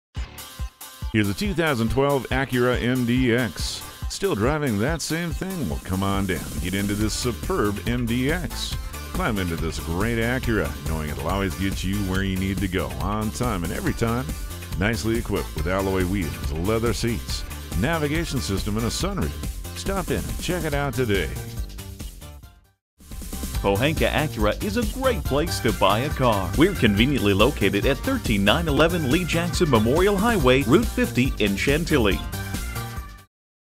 2012 Acura MDX Fairfax Acura Washington-DC, MD AEB050058A - SOLD
Year: 2012
Make: Acura
Model: MDX
Trim: Technology
Engine: 3.7L V6 SOHC VTEC 24V
Transmission: 6-Speed Automatic
Color: Black
Mileage: 30855
Stock #: AEB050058A
AWD! Nav! Are you still driving around that old thing? Come on down today and get into this superb 2012 Acura MDX! Climb into this great Acura MDX, knowing that it will always get you where you need to go, on time, every time.

Address:
13911 Lee Jackson Hwy Chantilly VA